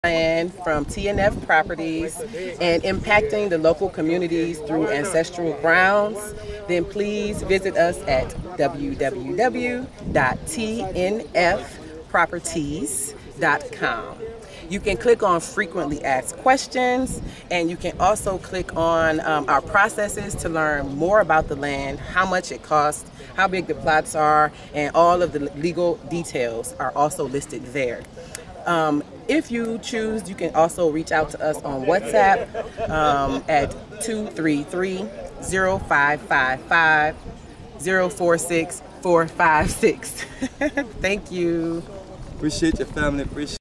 TNF Properties for info on Ancestral Grounds Community Development in Elmina, Ghana May 2023 Journey
Tour of the Ancestral Grounds Community Development in Elmina, Central Region of Ghana. Truth and Faith Enterprise are a conglomerate of real estate and repatriation services.